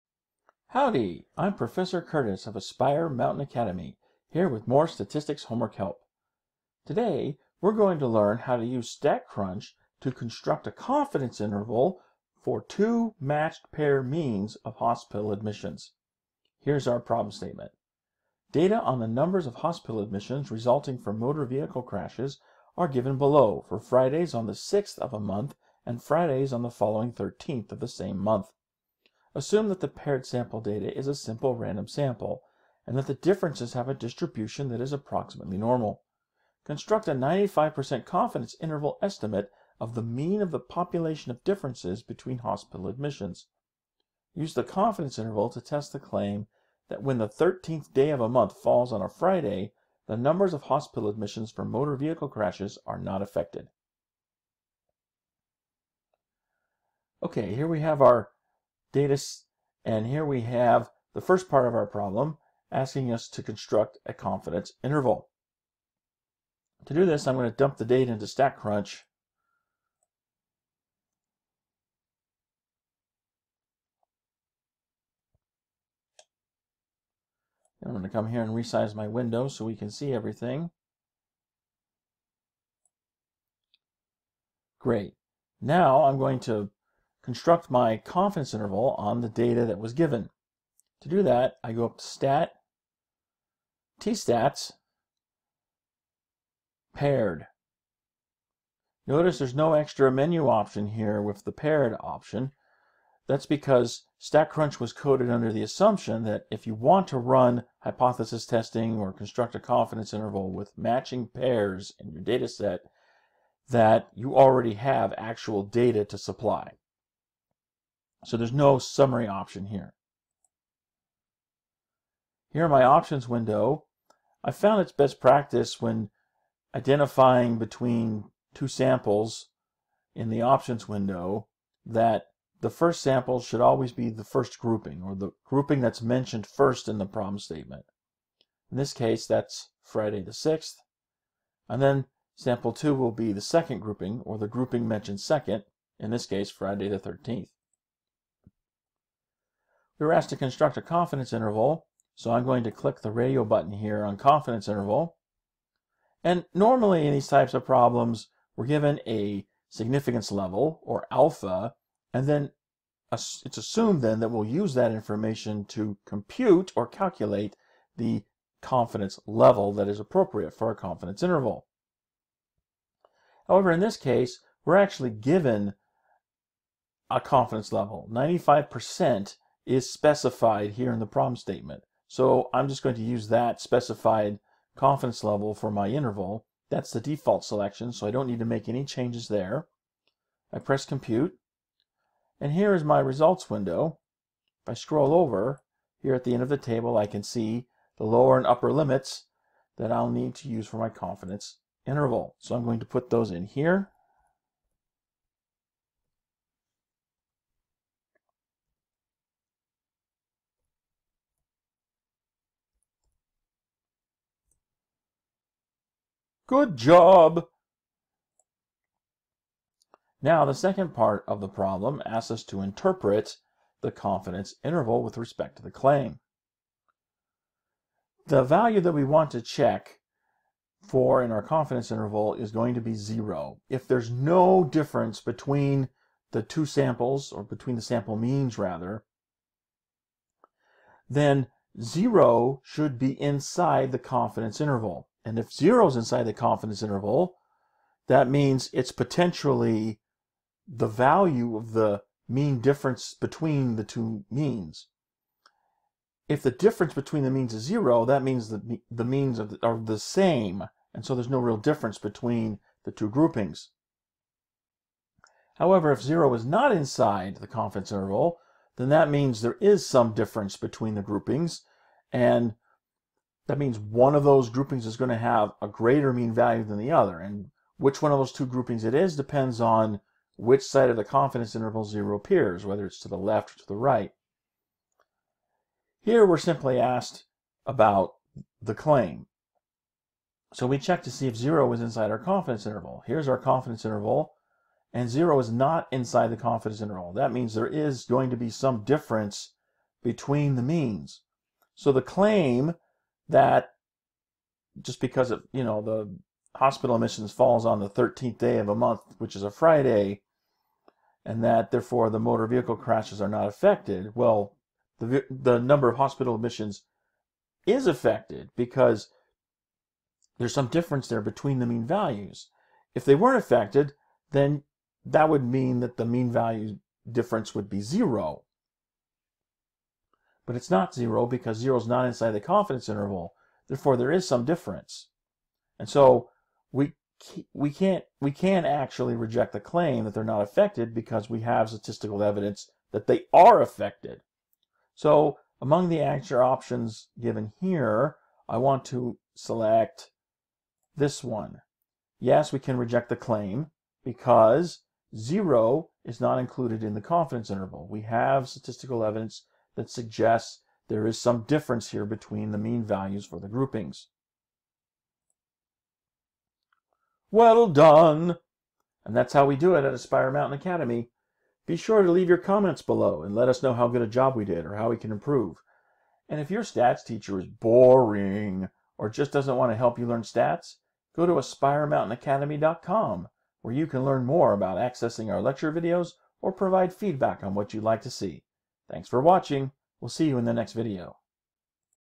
Using StatCrunch to construct a CI for two matched pair means of hospital admissions
In this video, Professor Curtis demonstrates how to use StatCrunch to construct a confidence interval for two matched pair means of hospital admissions (MyStatLab ID# 9.3.13-T).